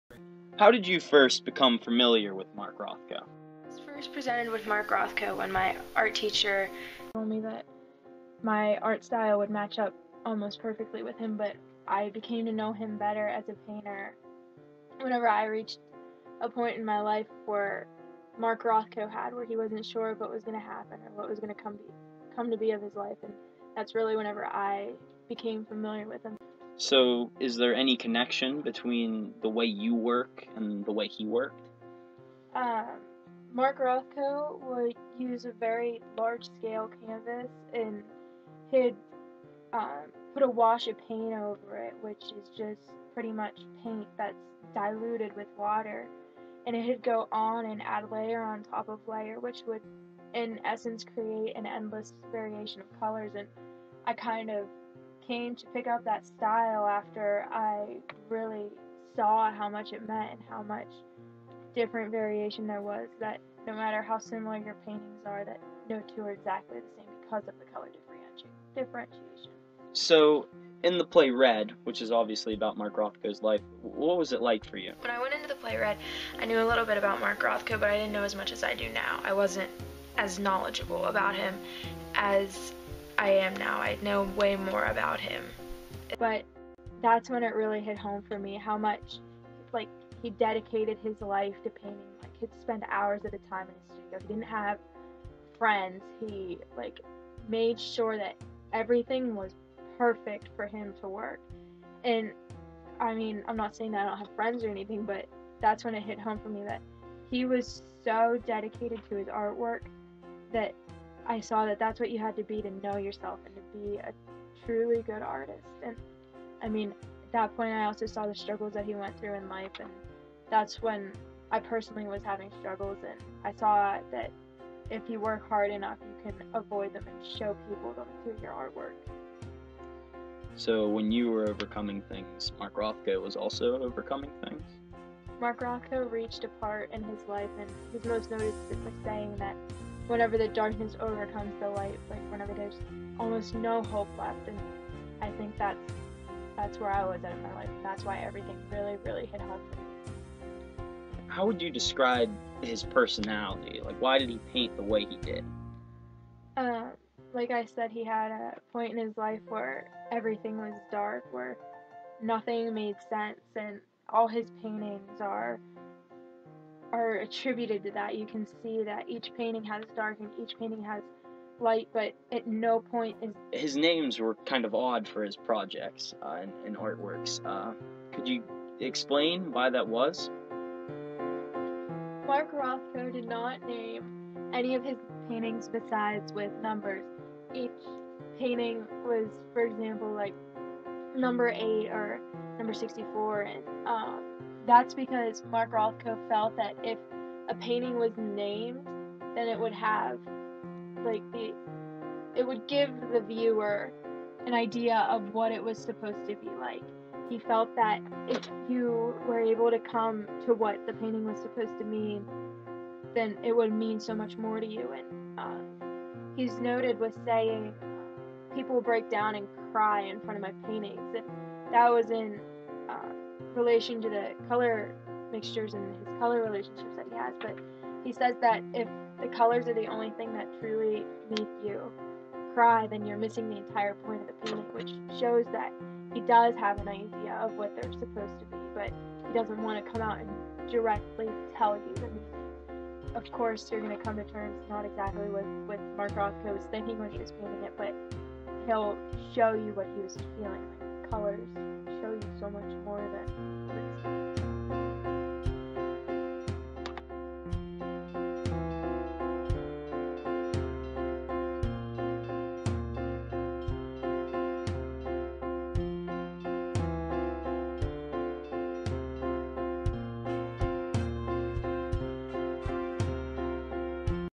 Mark Rothko
Clarion Art History creates a video about the amazing artist Mark Rothko. No copyrights infringement.